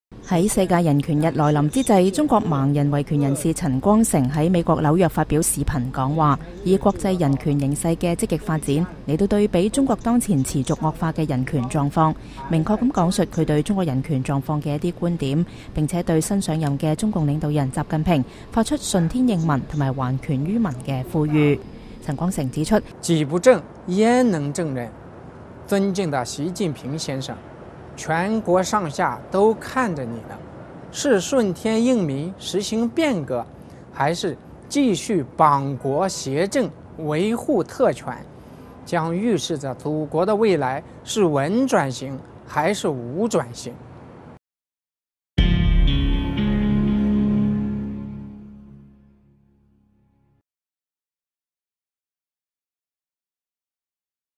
陳光誠發表世界人權日視頻講話
世界人權日來臨之際，中國盲人維權人士陳光誠在美國紐約發表視頻講話，以國際人權形勢的積極發展來對比中國當前持續惡化的人權狀況，明確闡述他對中國人權狀況的一些觀點，並且對新上任的中共領導人習近平發出"順天應民"和"還權於民"的呼籲。